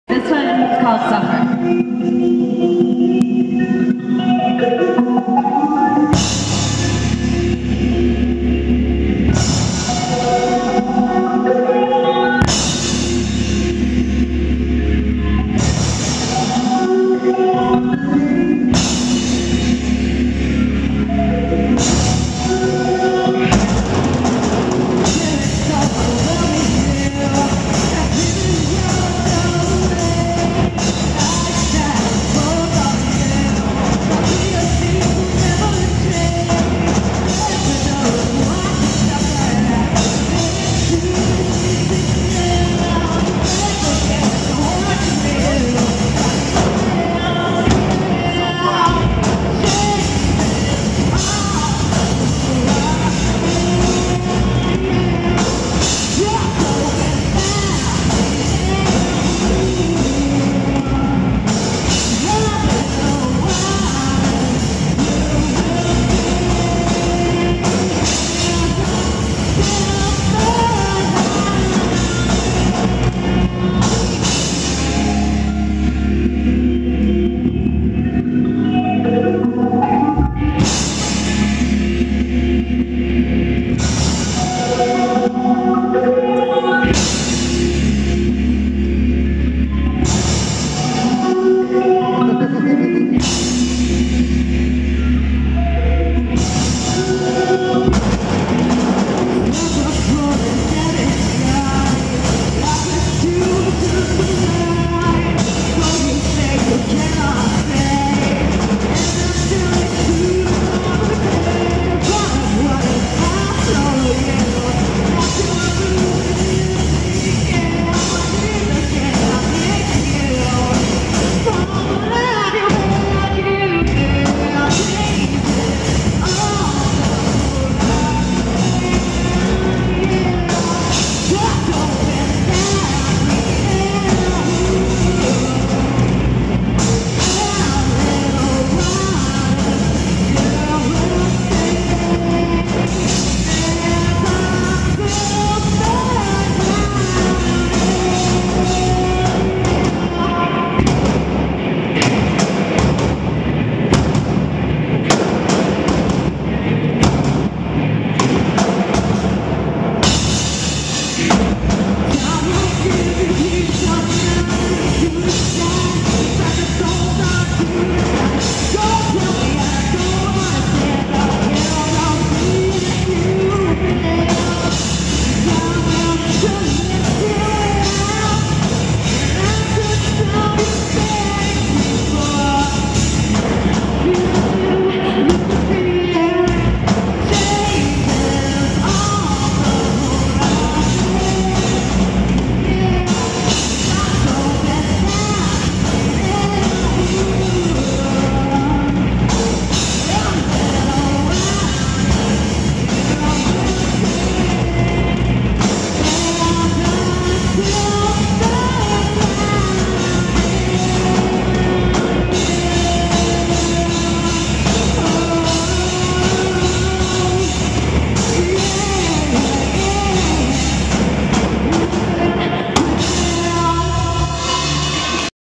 Turning Point At Open Mic Night (The Brick Church)
A local band named Turning Point performing one of there songs at an open mic night.